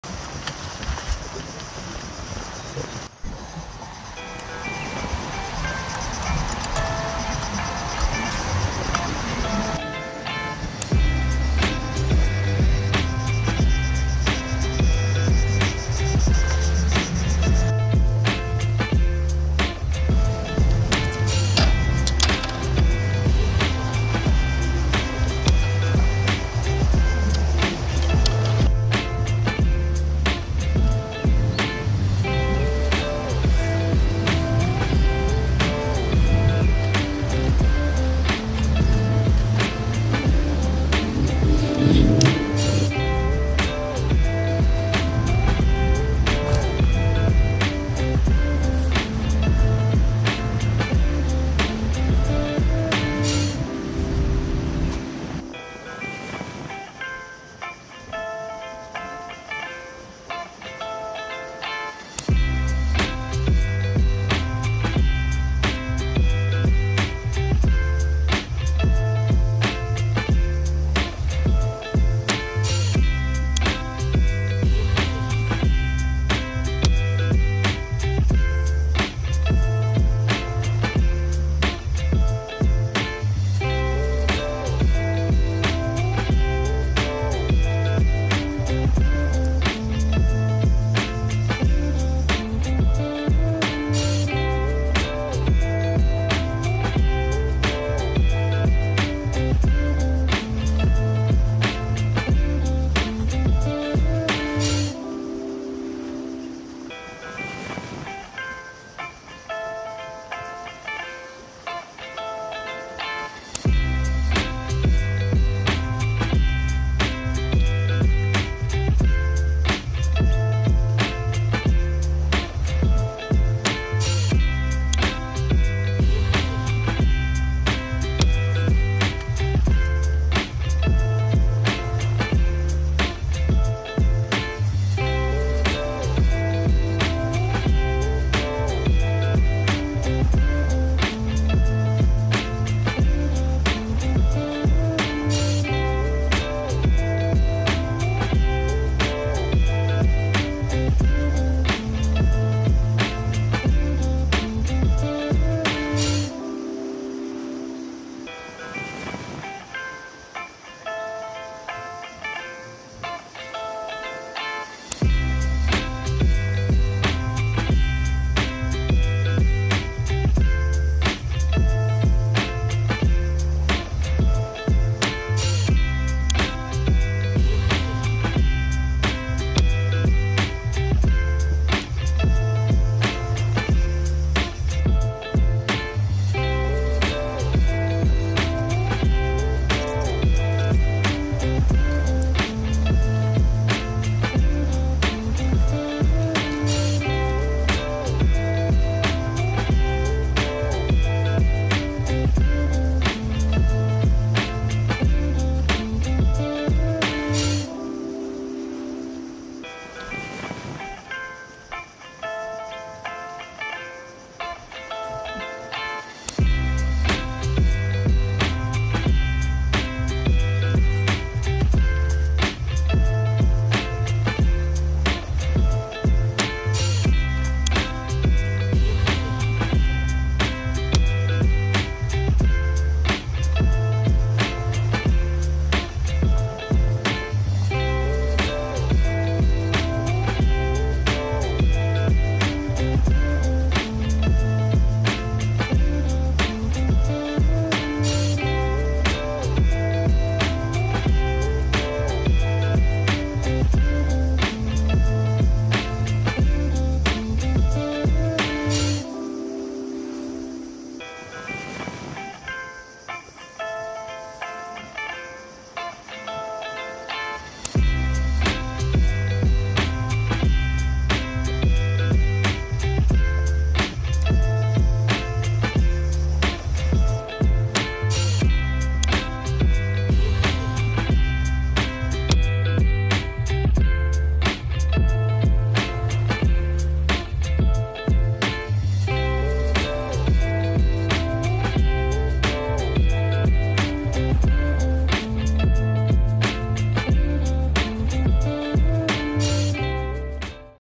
VIRAL?! Dome Shelter Mountainhiker | Khemah dengan Pintu & Jaring
Shelter inovatif dan selesa ini telah menarik perhatian penggemar aktiviti luar dan pencari pengembaraan.